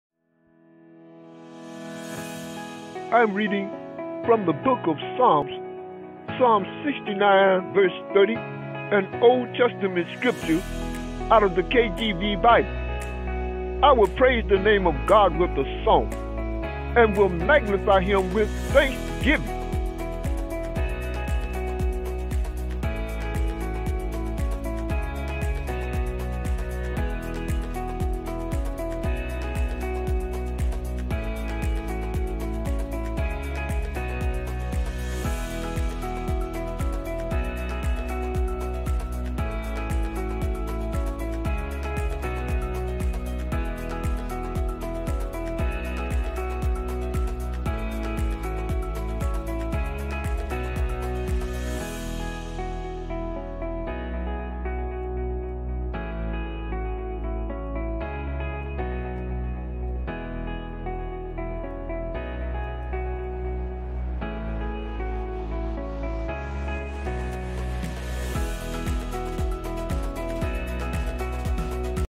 PSALMS 69 30
PSALMS 69 30  I will praise the name of God with a song, and will magnify him with thanksgiving. Praise his name with A song and make him great with praise